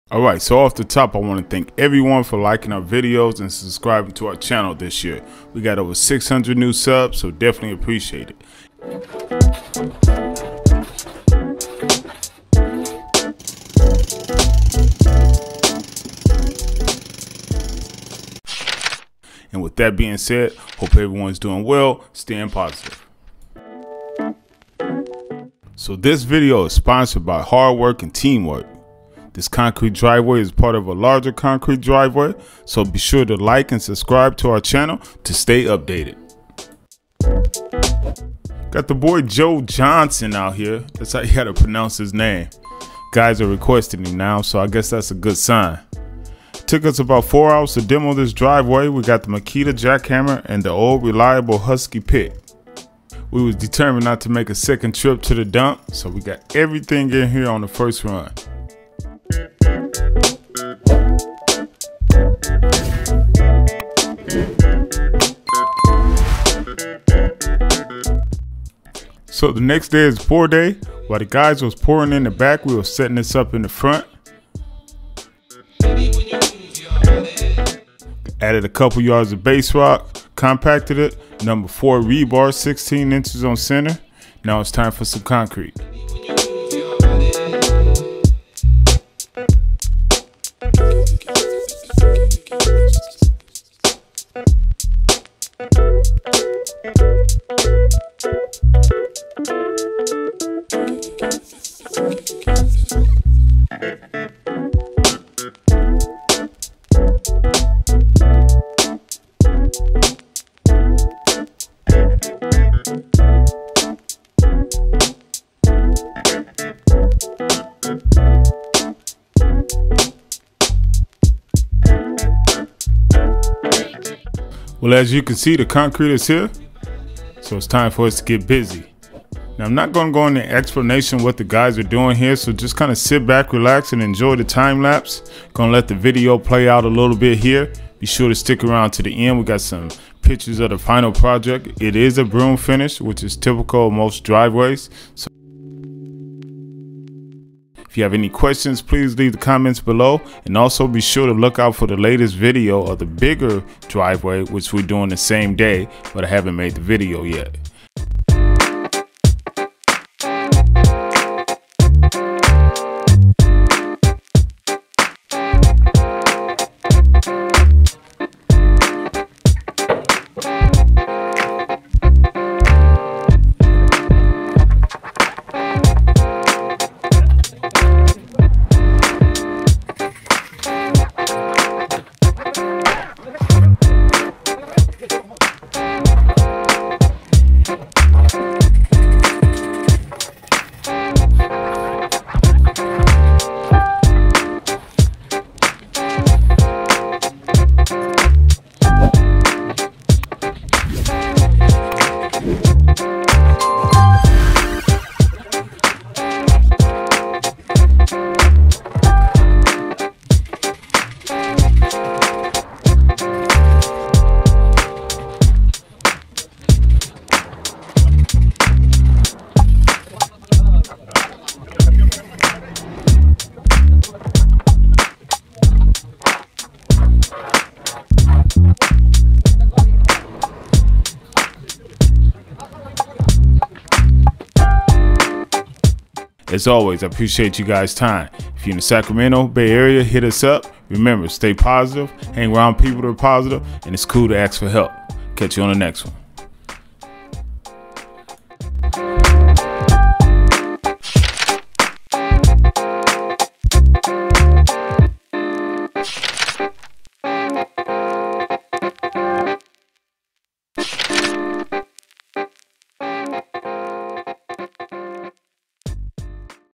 SEE What It Takes To Demo & Pour CONCRETE Driveway | All Access
See What It Takes To Demo And Pour Concrete Driveway. All Teamwork and Muscle over here.

SOCIAL MEDIA LINKS:

How To demo and pour concrete driveway bay area